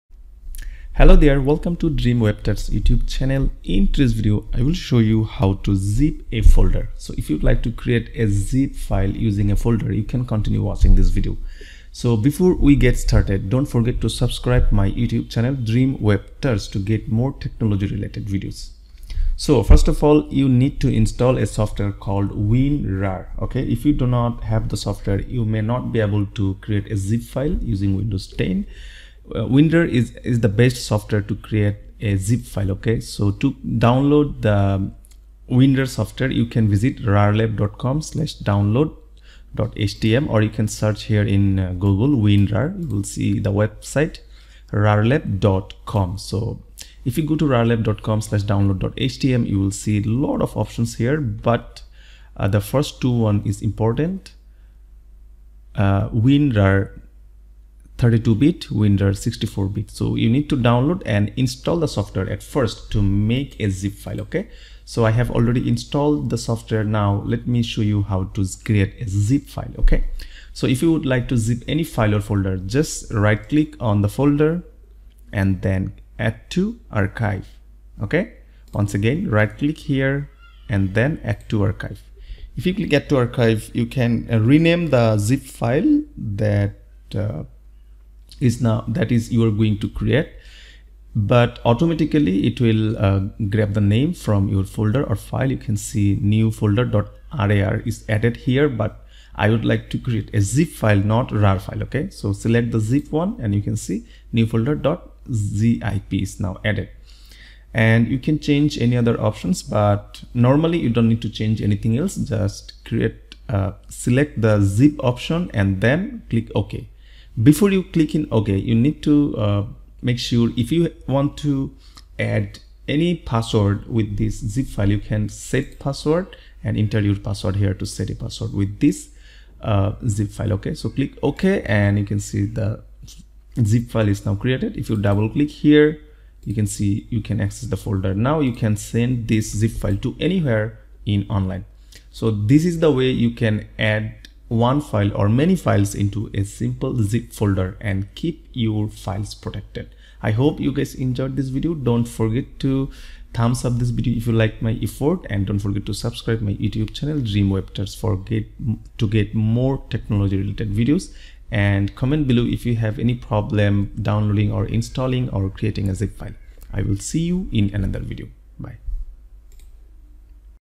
How to Create a Zip File - Using Winrar
Guys, I will show you How to Zip a Folder using WinRAR. You can easily create a zip file using WinRAR. I have explained this in detail here in this video. You need to right-click on a folder and click add to archive. Then you can rename, select the archive type and set a password from the options panel.